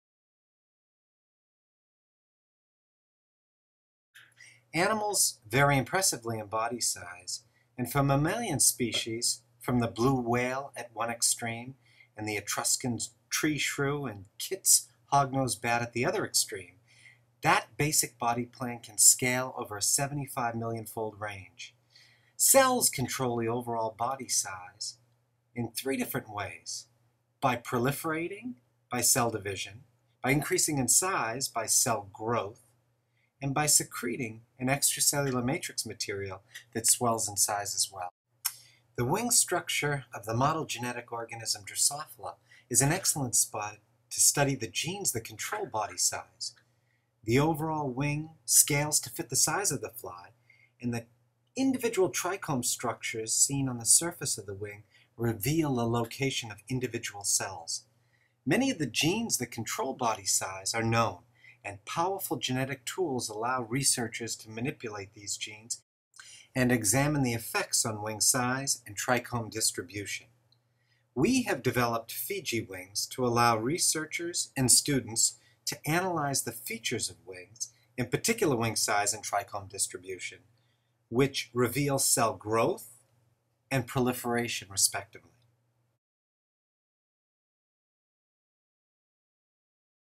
FijiWings Intro
Brief conceptual introduction to the use of the fruit fly Drosophila to study cell growth and proliferation and how Fijiwings can facilitate such studies.